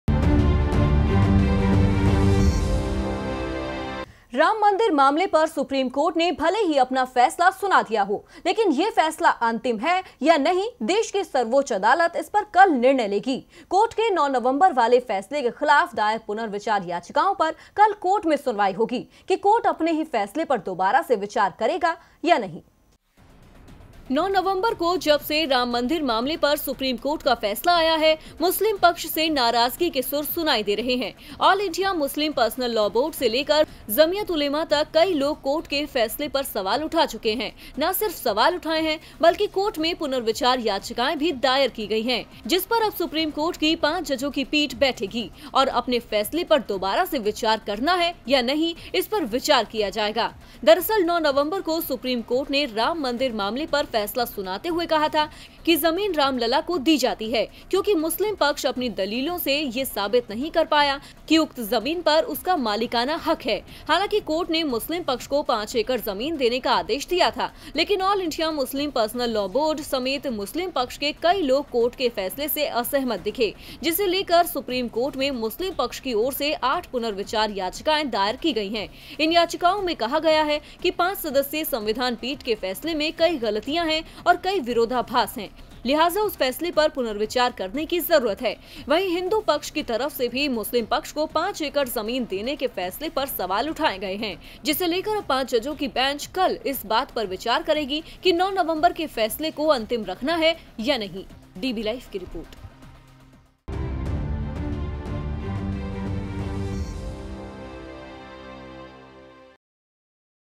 पुनर्विचार याचिकाओं पर सुनवाई करेगा कोर्ट | Ram Mandir मामले में कल SC में सुनवाई
राम मंदिर मामले पर सुप्रीम कोर्ट ने भले ही अपना फैसला सुना दिया हो...लेकिन ये फैसला अंतिम है या नहीं...देश की सर्वोच्च अदालत इसपर कल निर्णय लेगी...कोर्ट के 9 नवंबर वाले फैसले के खिलाफ दायर पुनर्विचार याचिकाओं पर कल कोर्ट में सुनवाई होगी...कि कोर्ट अपने ही फैसले पर दोबारा से विचार करेगा या नहीं...